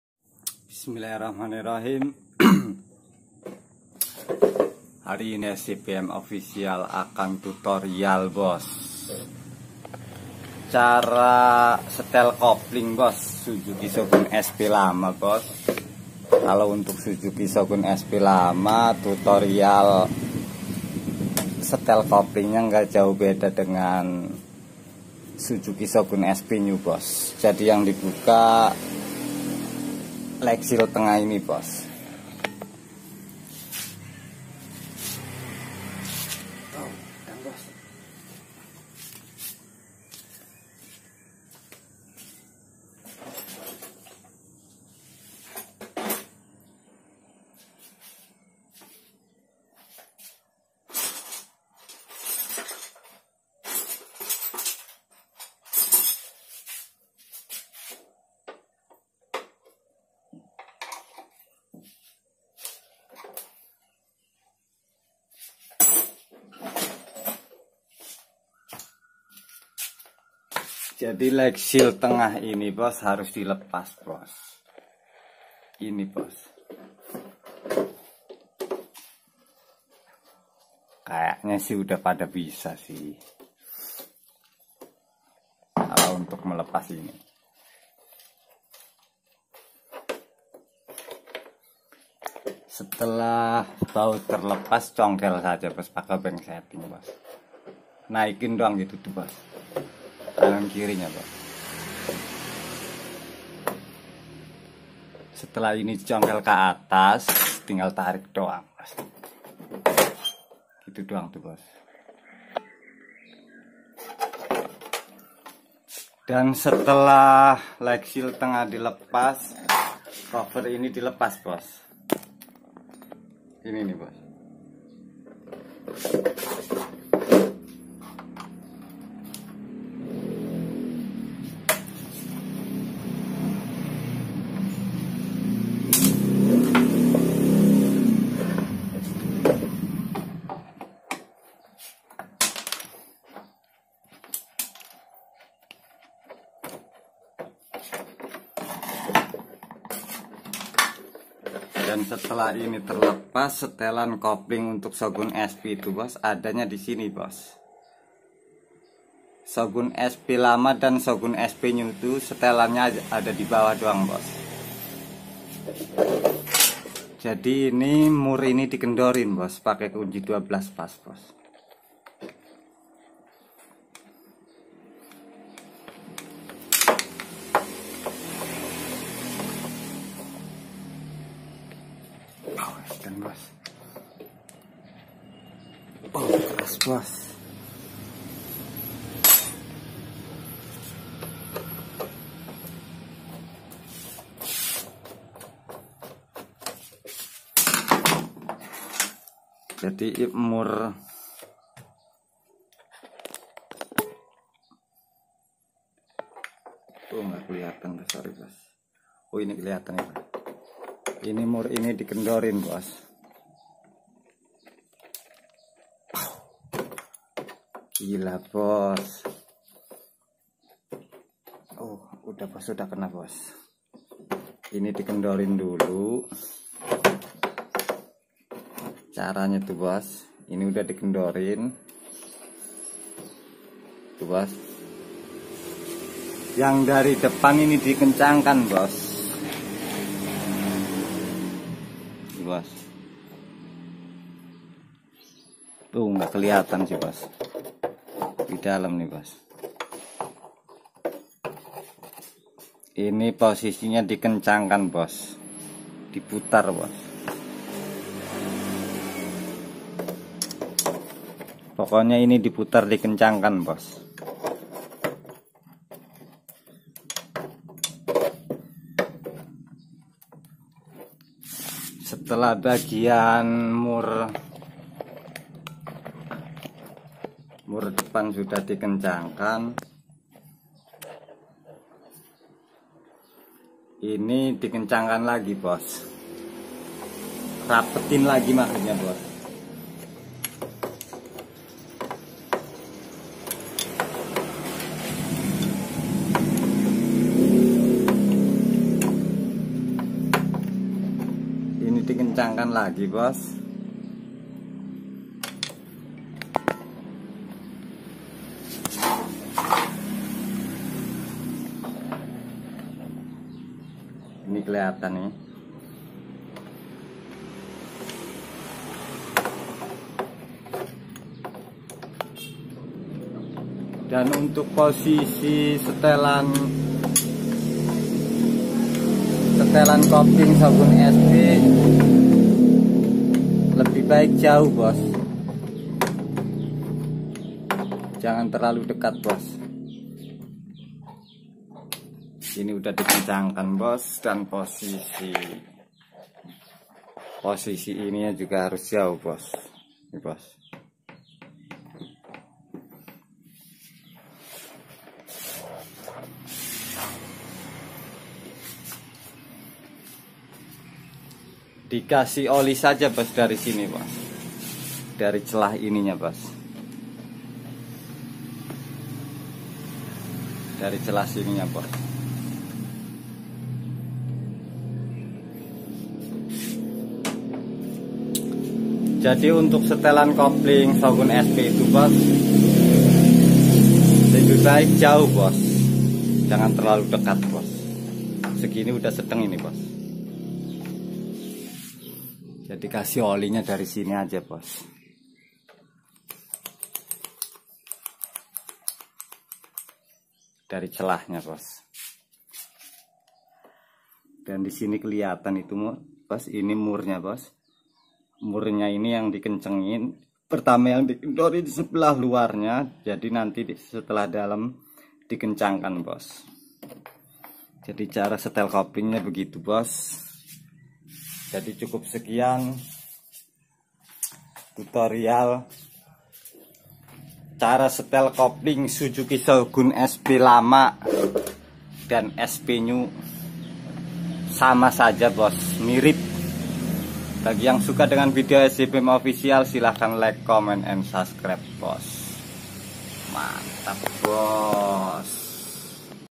cara stel kopling suzuki shogun Sp old dan Sp New sama saja bos
Cara stel kopling suzuki shogun Sp old dan Sp New memang sama saja bos simak video berikut dari sjbm oficial bagi yang suka dengan video sjbm oficial silahkan like comment n scrb bos terimakasih selamat menyaksikan